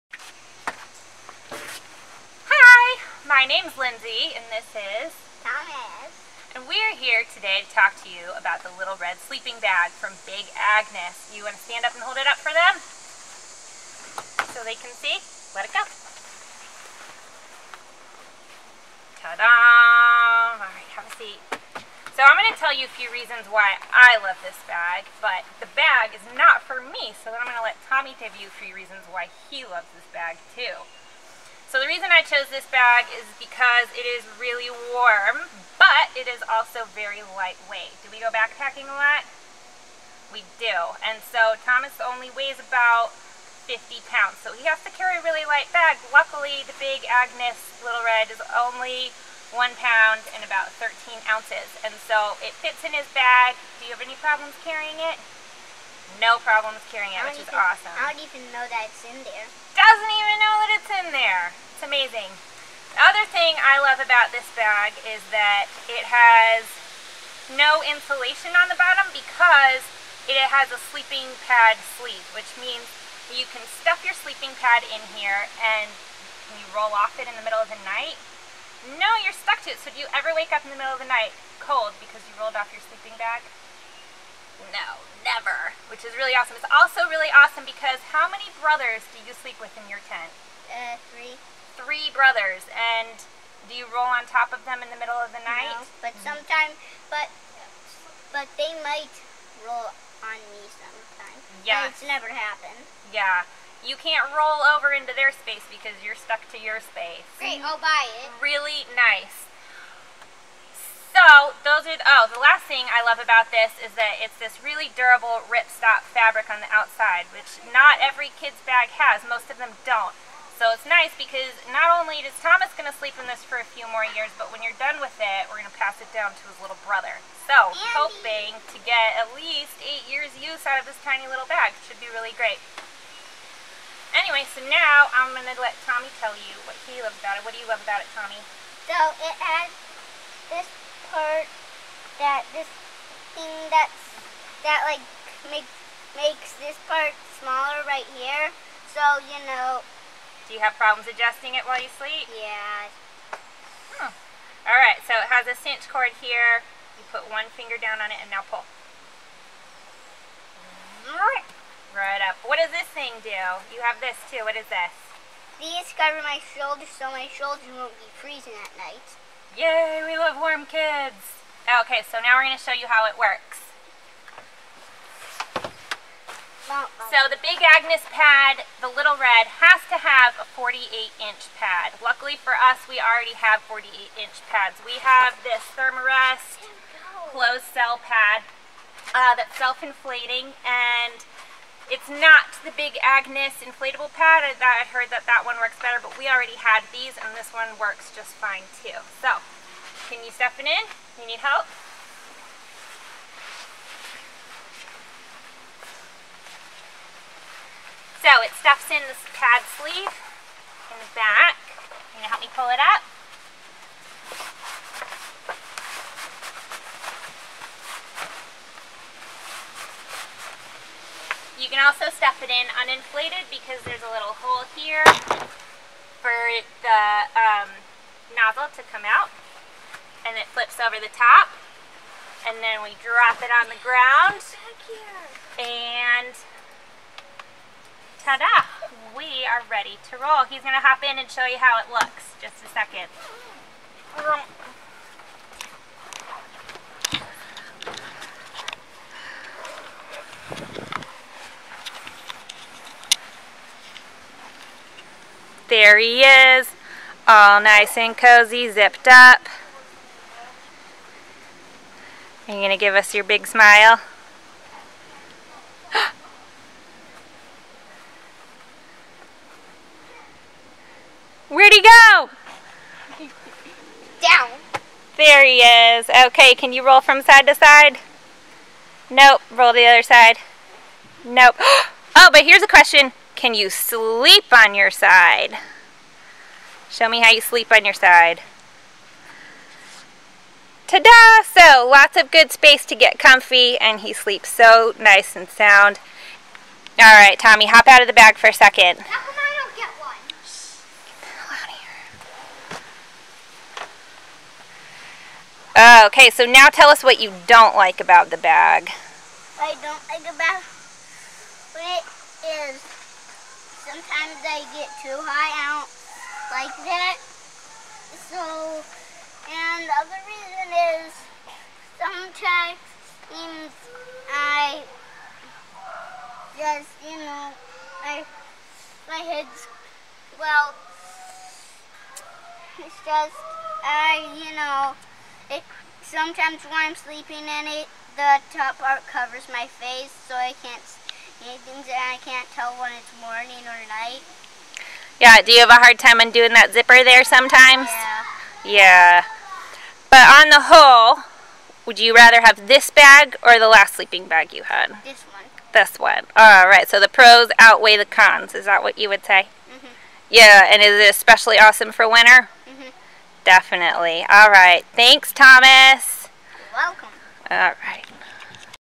Gear Review of the Big Agnes Little Red sleeping bag
Thomas and I say what we love and don't love about this ultra warm, kids bag.